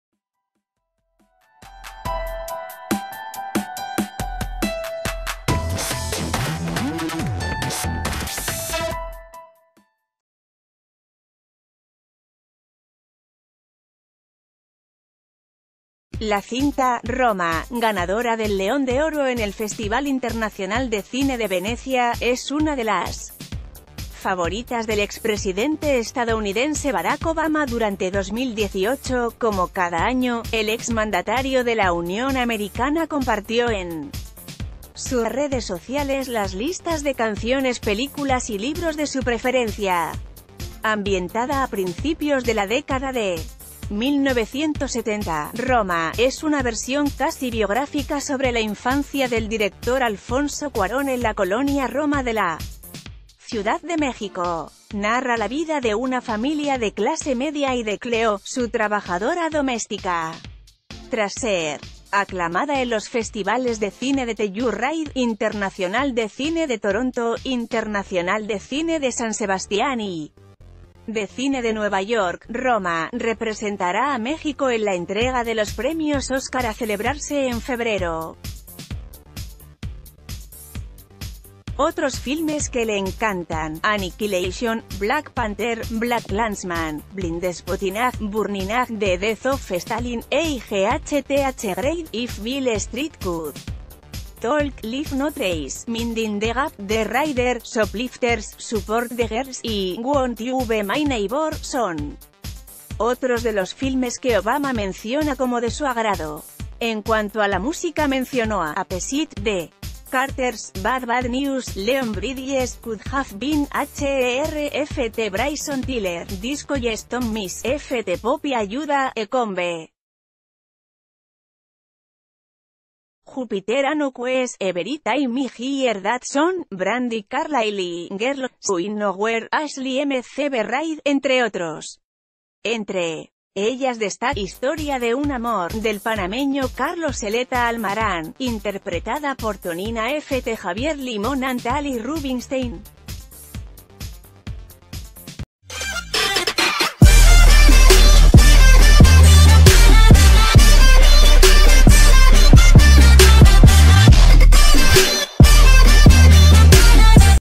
Roma, una de las películas favoritas de Barack Obama
La cinta “Roma”, ganadora del León de Oro en el Festival Internacional de Cine de Venecia, es una de las favoritas del ex presidente estadounidense Barack Obama durante 2018.Como cada año, el ex mandatario de la Unión Americana compartió en sus redes sociales las listas de canciones, películas y lib...